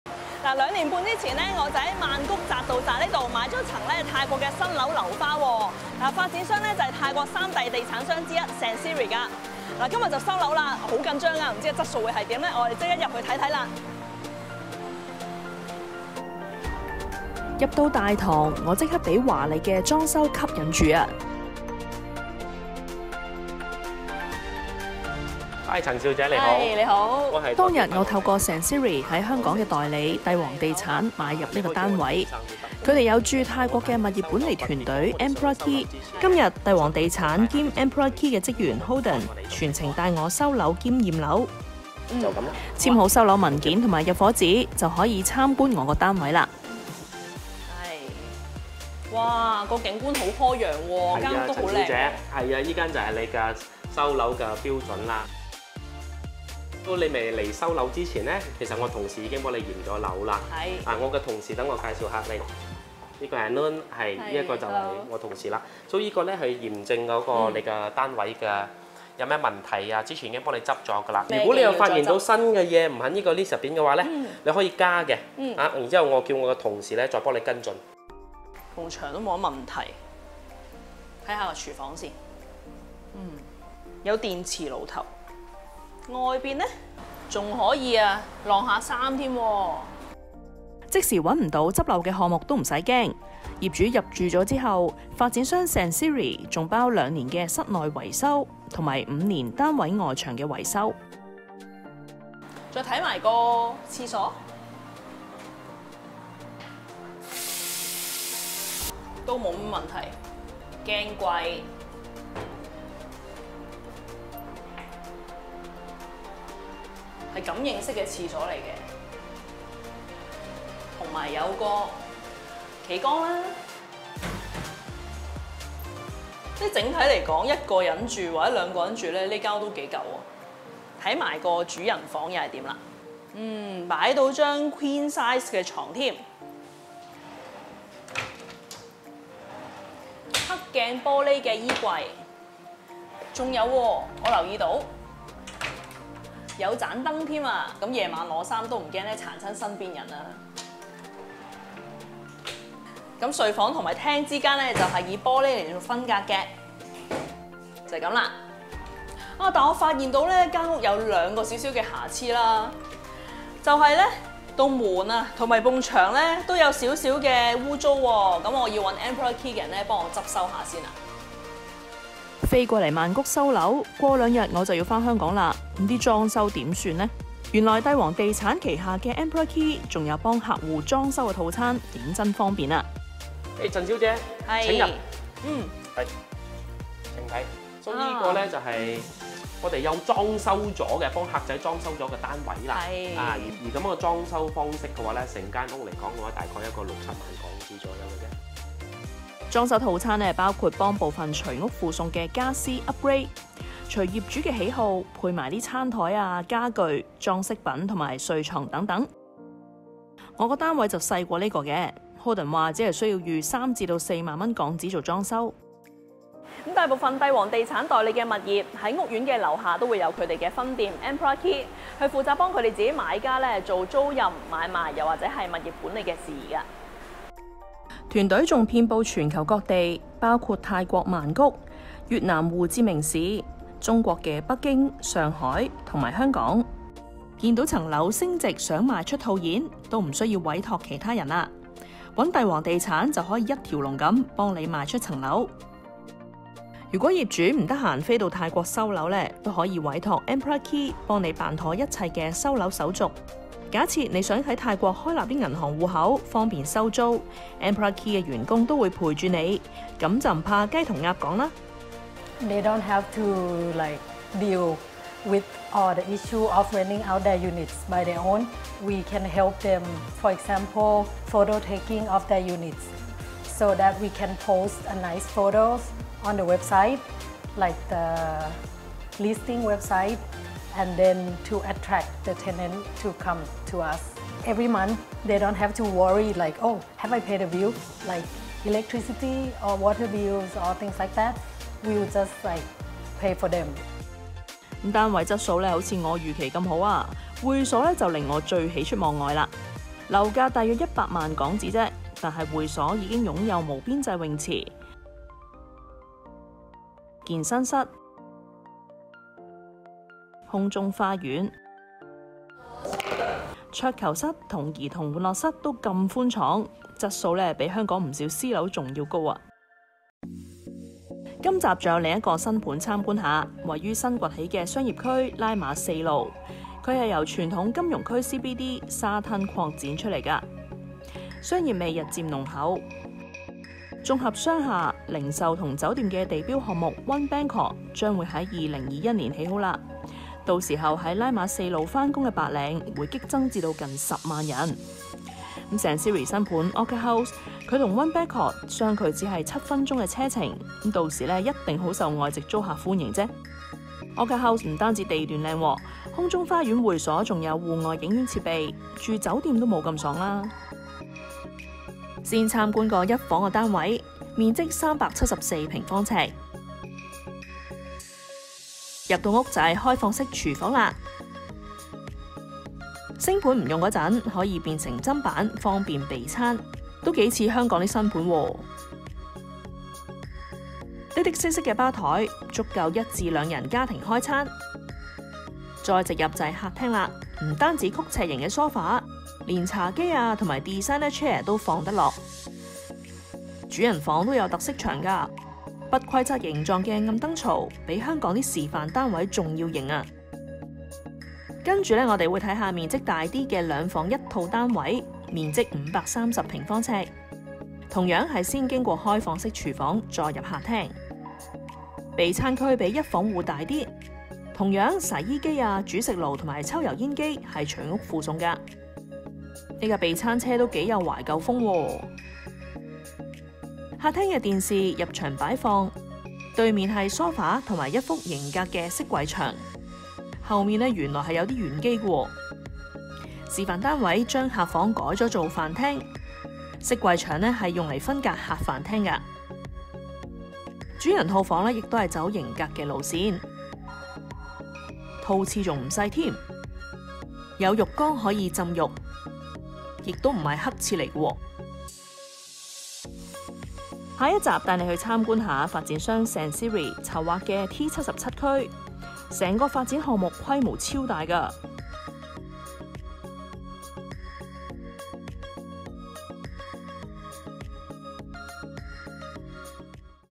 Golden Emperor ------ One-Stop Services Overseas Property Agency
Crystal Chan, former news anchor of TVB in Hong Kong, brings us to feel the services which provided by Golden Emperor and our subsidiary Emperor Key